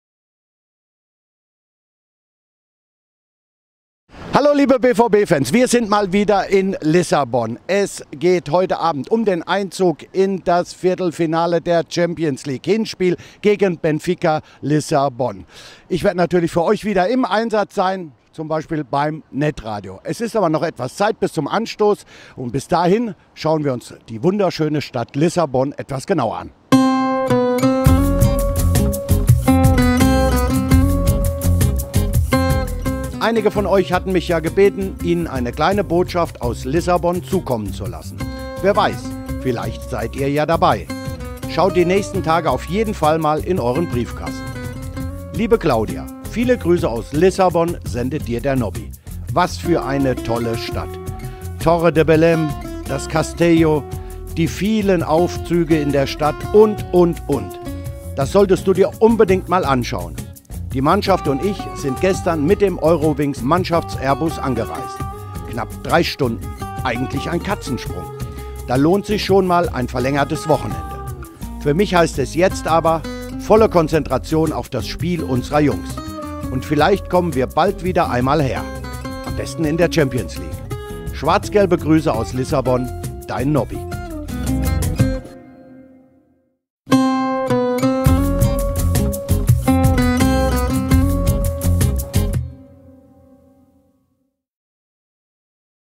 Post von Nobby aus Lissabon | SL Benfica - BVB
Liebesgrüße aus Lissabon: Zum Valentinstag war unser Nobby in der portugiesischen Hauptstadt unterwegs und hat Postkarten an die BVB-Fans geschrieben. Vielleicht habt Ihr ja auch eine bekommen...